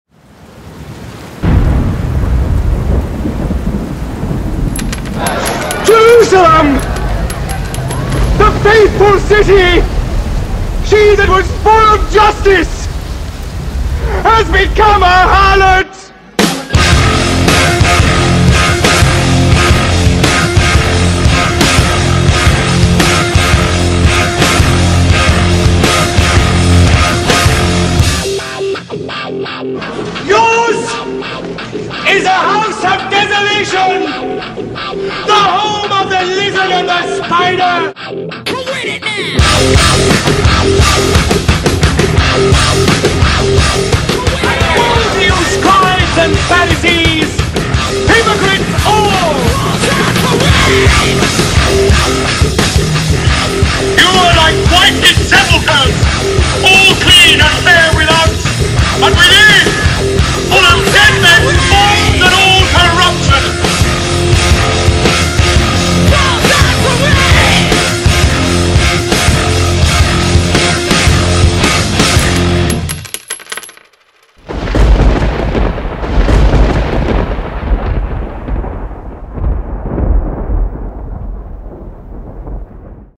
FeelTheBern and VOTE March 15 in Ohio
There was another socialist Jew who Bernie Sanders sounds a lot like. Vote on March 15 in Ohio for Bernie Sanders for President of the United States.